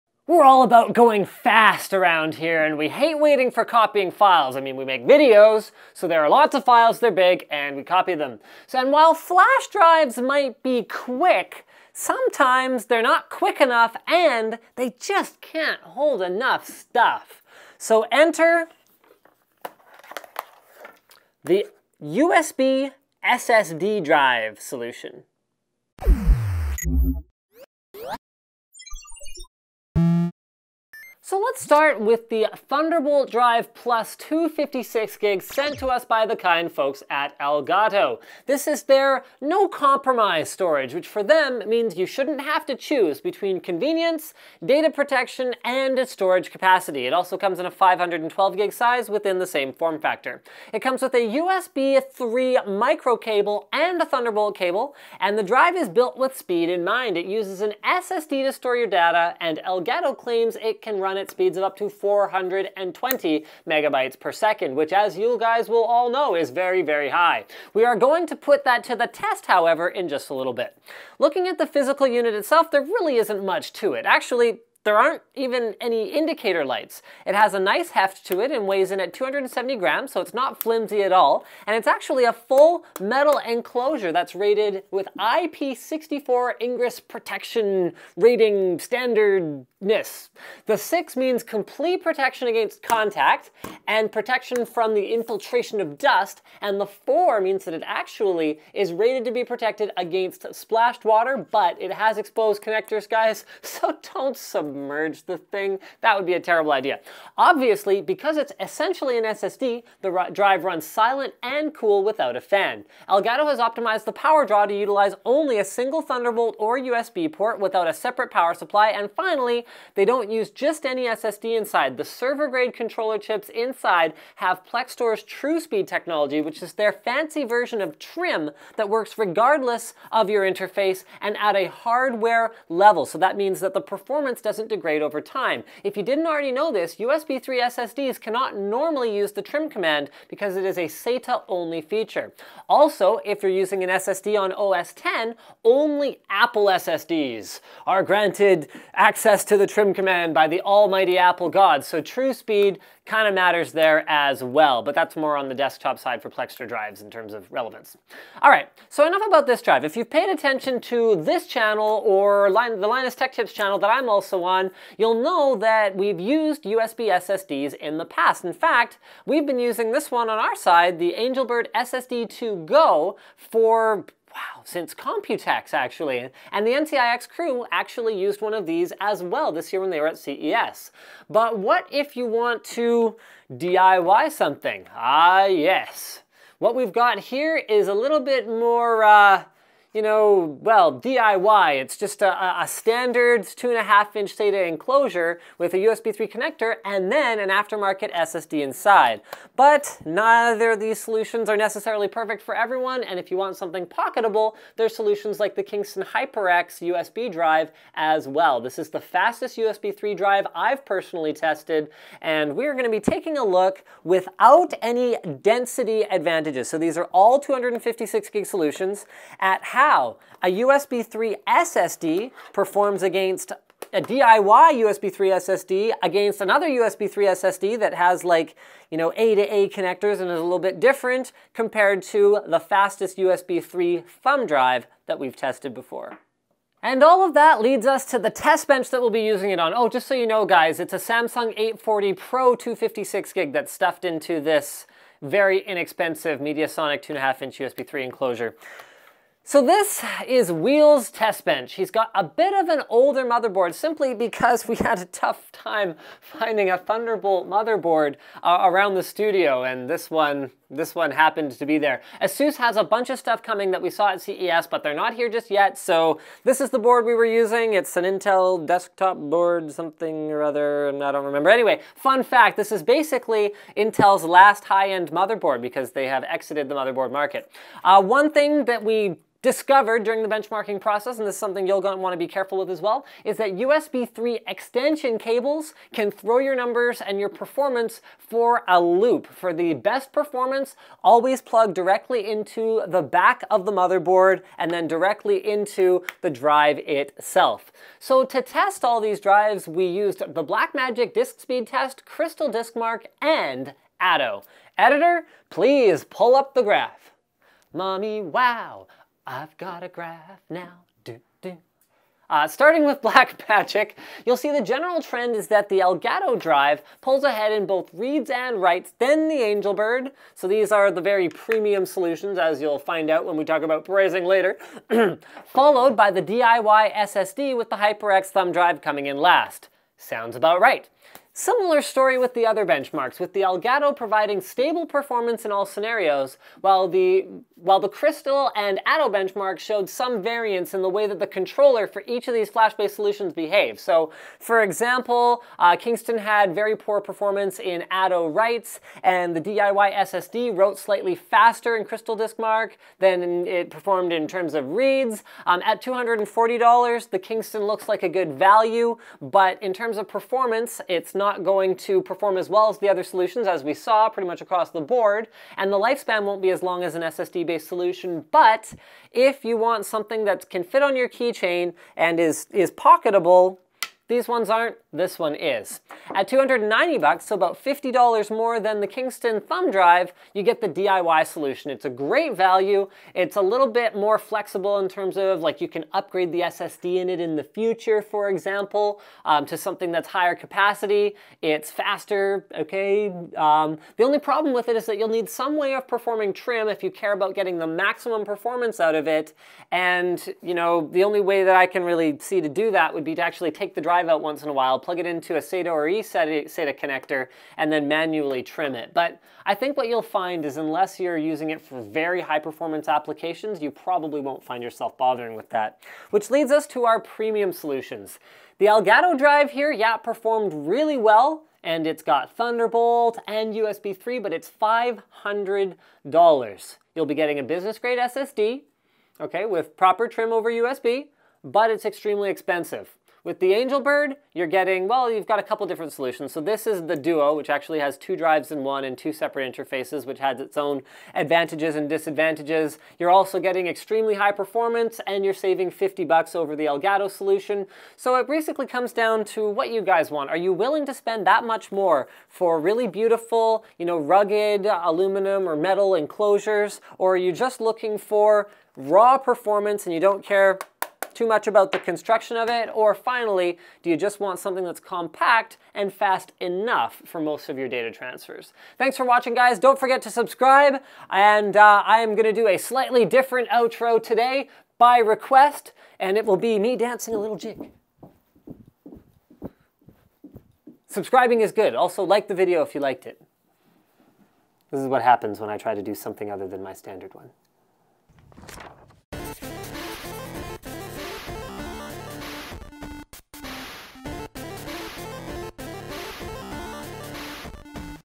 USB 3.0 SSD Comparison Showdown
Find out what high-speed solution is best for you when transporting large files on the go.  Has flash storage has become cheap enough and is USB 3.0 fast enough for us to carry around USB attached SSD drives instead of pen drives?